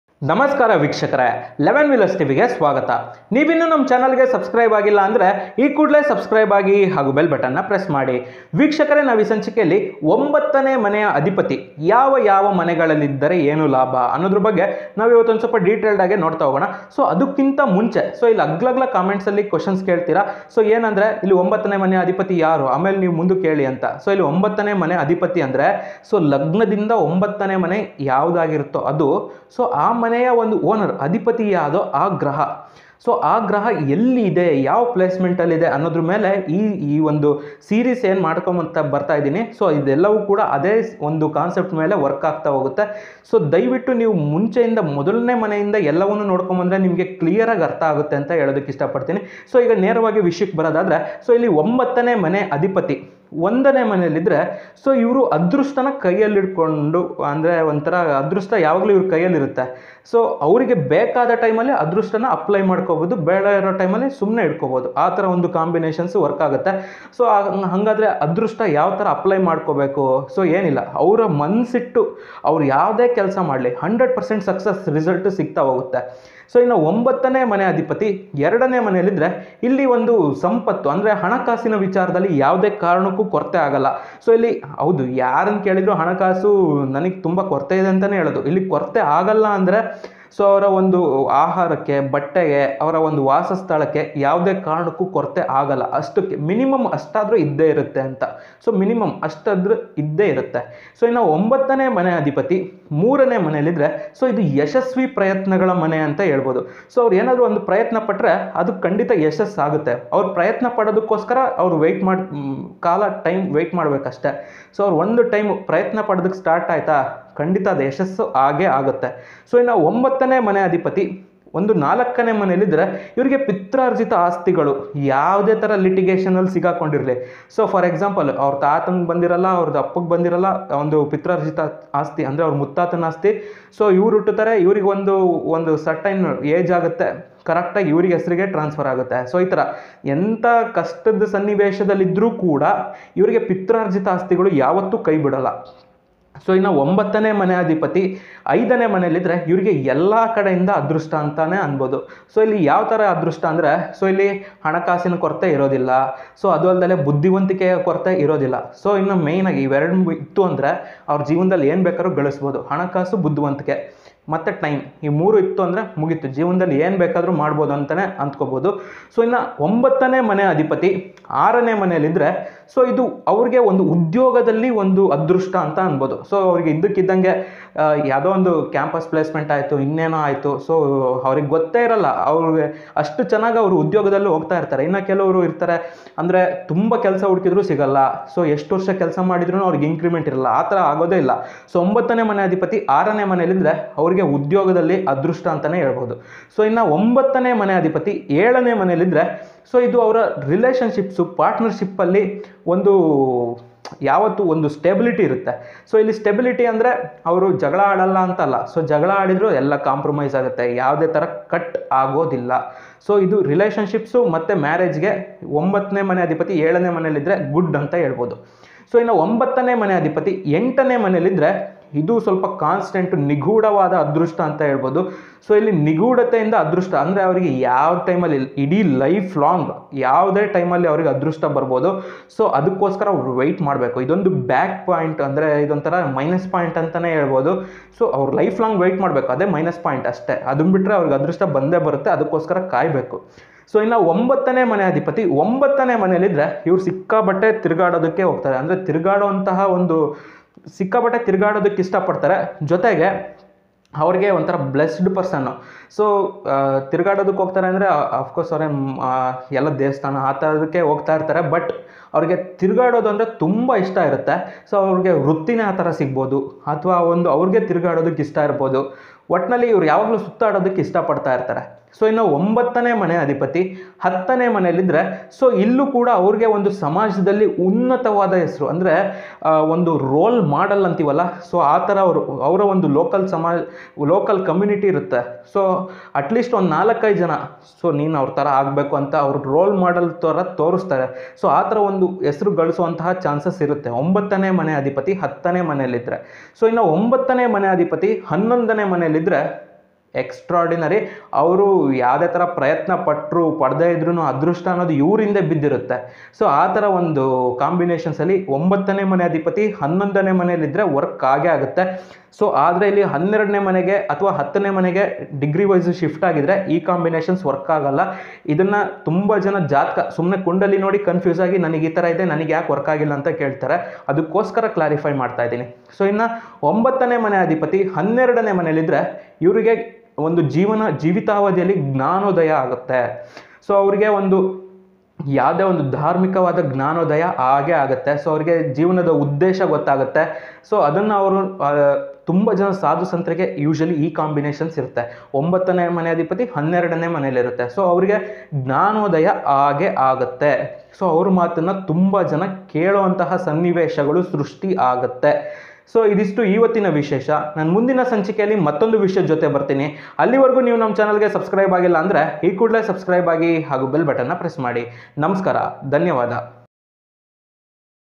What if 9th house lord in 9th house | what are the effects of 9th house lord in different houses
This video is made for technical analysis of astrology and to move on to scientific path of astrology basics of Hindu culture  and tradition